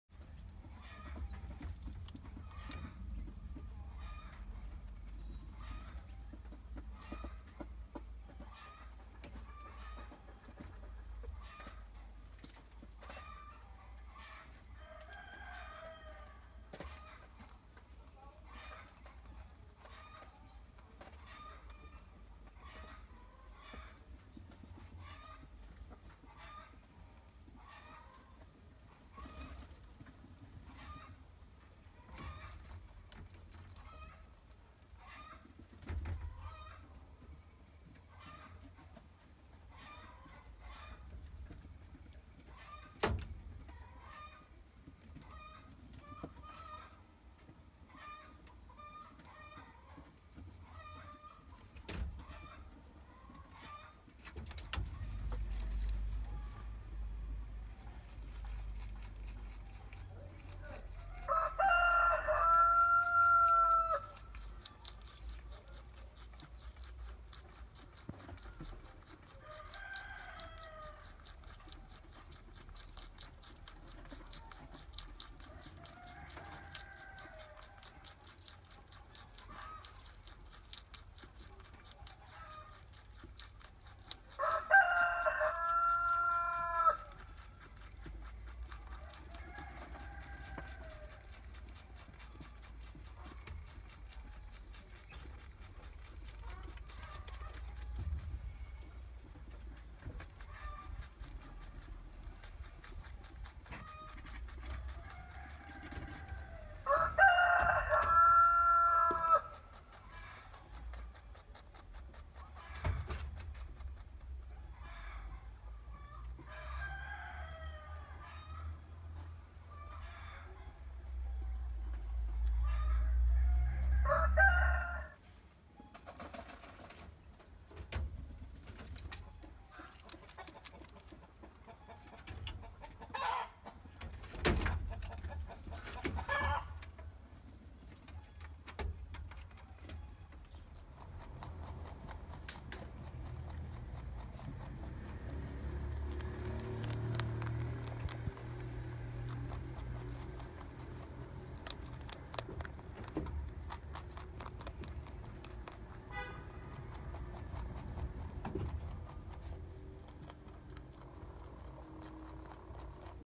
Tavşanlar En Çok Neyi Sever
rabbits bunny bunnies Tavşanların en sevdiği yiyecekler sizce nelerdir? Havuç mu Portakal mı sebzeler mi meyveler mi.. Tavşanlar En Çok Neyi Sever? Sizde video yorum bölümünde tavşanlar en çok neyi sever paylaşabilirsiniz.